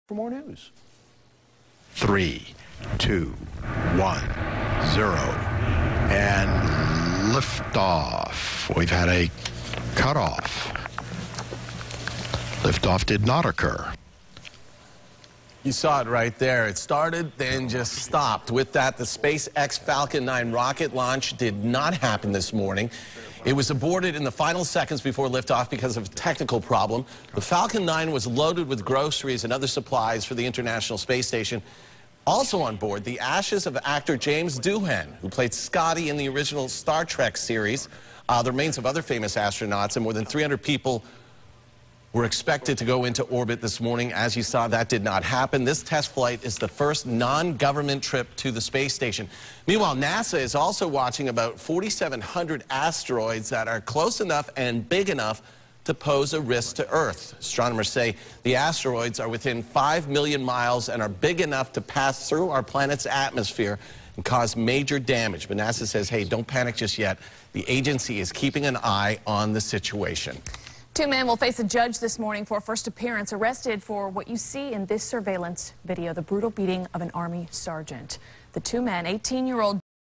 SpaceX rocket launch aborted at the last second... literally
The countdown reached all the way to practically zero Saturday morning for the SpaceX Falcon 9 rocket. The engine ignition sequence started up, but there was an automatic shutdown by on-board computers. So instead of blasting off on a delivery mission to the space station, the rocket stayed on its launch pad amid a cloud of engine exhaust.